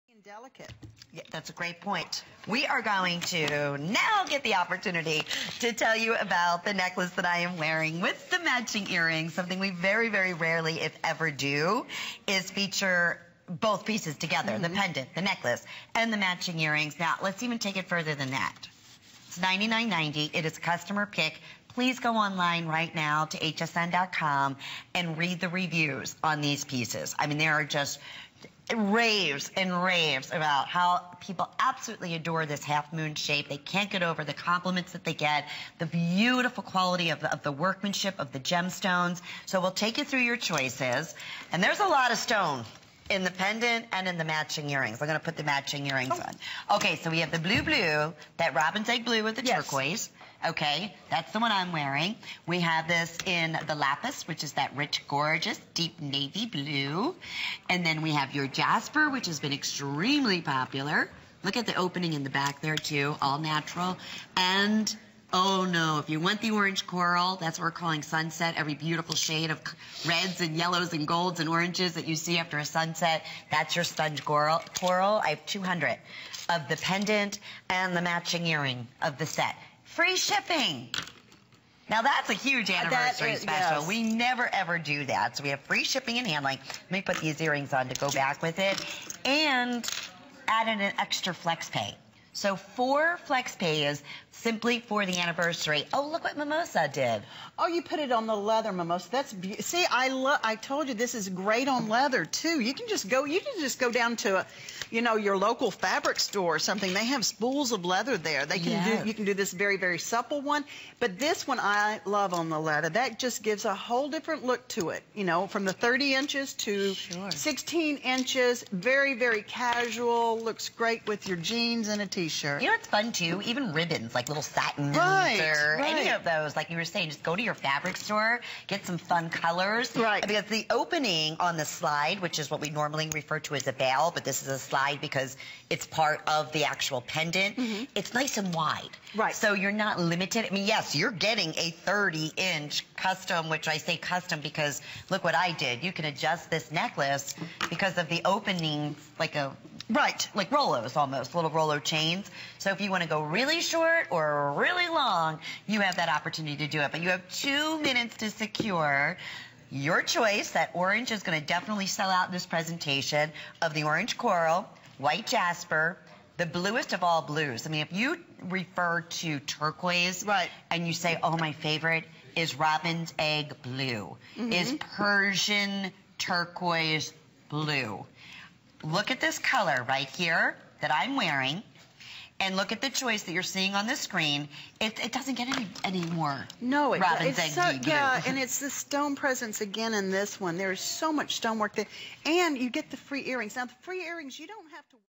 Studio Barse Sponge Coral Pendant and Earring Set
Studio Barse Orange Sponge Coral Bronze HalfMoon Pendant and Earring Set
[Value Statement]

Design Information

    Jewelry set includes (1) pendant with chain and (1) pair of earrings